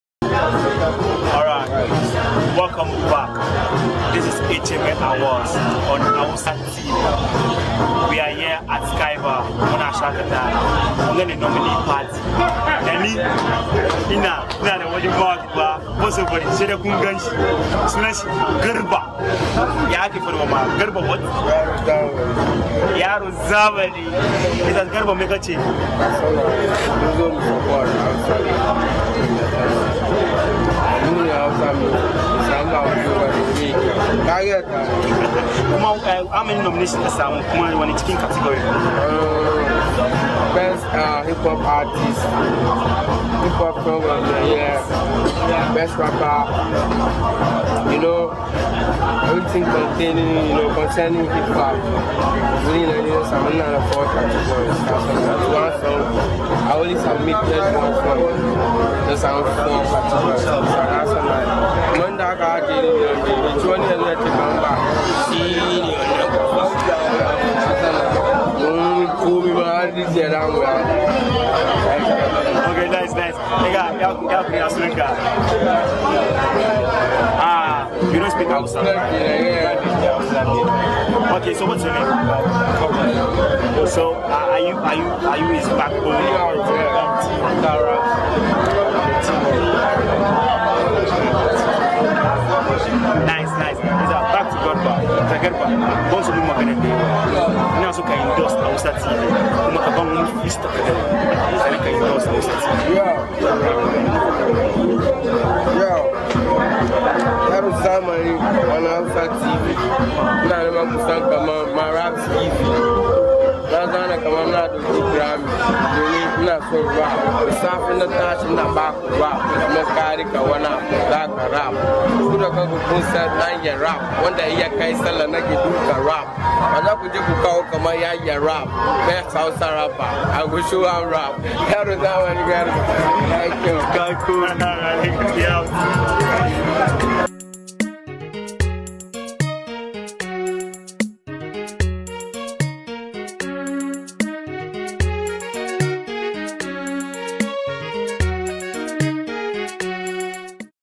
Gariba Yaronzamani at the Hausa Music Award Nominees Party, Sky Bar Lounge & Night Club
Interview with Yaronzamani Vs Gariba at the Hausa Music Award Nominees Party.

For Latest News, Education, Entertainment, Sports, Culture and Tourism